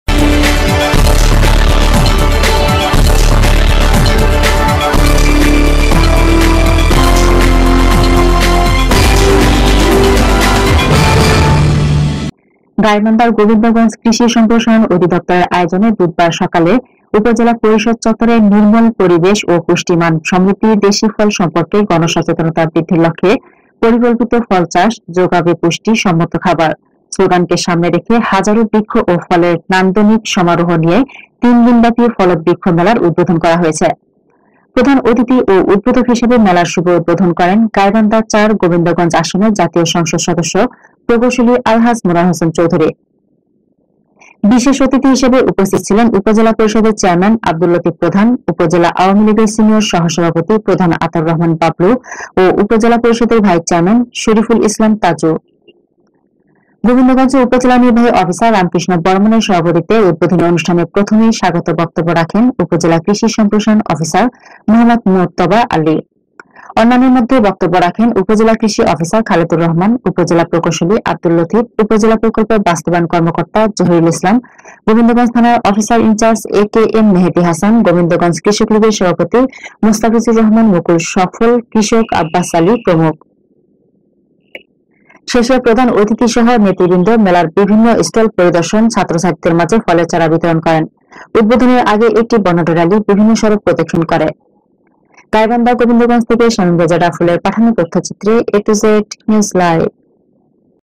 গোবিন্দগঞ্জে ৩দিন ব্যাপী ‘ফলদ বৃক্ষ মেলা’র উদ্বোধন করা হয়েছে|Gaibandha|a2znewslivetv
গাইবান্ধার গোবিন্দগঞ্জ কৃষি সম্প্রসারন অধিদপ্তরের আয়োজনে বুধবার সকালে  উপজেলা পরিষদ চত্বরে নির্মল পরিবেশ ও পুষ্টিমান সমৃদ্ধ দেশী ফল সম্পর্কে গণসচেতনতা বৃদ্ধির লক্ষ্যে “ পরিকল্পিত ফল চাষ-যোগাবে পুষ্টি সম্মত খাবার ” শ্লোগানকে সামনে রেখে হাজারো বৃক্ষ ও ফলের নান্দনিক সমারোহ নিয়ে তিনদিন ব্যাপী ‘ফলদ বৃক্ষ মেলা”র উদ্বোধন করা হয়েছে। প্রধান অতিথি ও উদ্বোধক হিসেবে মেলার শুভ উদ্বোধন করেন গাইবান্ধা-৪ গোবিন্দগঞ্জ আসনের জাতীয় সংসদ সদস্য প্রকৌশলী আলহাজ্ব মনোয়ার হোসেন চৌধুরী। বিশেষ  অতিথি হিসেবে উপস্থিত ছিলেন উপজেলা পরিষদের চেয়ারম্যান আব্দুল লতিফ প্রধান, উপজেলা আওয়ামীলীগের সিনিয়র সহসভাপতি প্রধান আতাউর রহমান বাবলু ও উপজেলা পরিষদের ভাইস চেয়ারম্যান শরিফুল ইসলাম তাজু। গোবিন্দগঞ্জ উপজেলা নির্বাহী অফিসার রামকৃষ্ণ বর্মনের সভাপতিত্বে উদ্বোধনী অনুষ্ঠানের প্রথমেই স্বাগত বক্তব্য রাখেন উপজেলা কৃষি সম্প্রসারণ অফিসার মোঃ মোরতবা আলী। অন্যান্যের মধ্যে বক্তব্য রাখেন উপজেলা কৃষি অফিসার খালেদুর  রহমান,  উপজেলা প্রকৌশলী আব্দুল লতিফ, উপজেলা প্রকল্প বাস্তবায়ন বর্মকর্তা জহিরুল ইসলাম, গোবিন্দগঞ্জ থানার অফিসার ইনচার্জ একেএম মেহেদী হাসান, গোবিন্দগঞ্জ কৃষকলীগের সভাপতি মোস্তাফিজার রহমান মুক,ু সফল কৃষক আব্বাস আলী. প্রমুখ।  শেষে প্রধান অতিধিসহ নেতৃবৃন্দ  মেলার বিভিন্ন স্টল পরিদর্শন ছাত্র-ছাত্রীদের মাঝে ফলের চারা বিতরন করেন। উদ্বোধনের আগে একটি বর্ণাঢ্য র‌্যালী বিভিন্ন সড়ক প্রদক্ষিণ করে।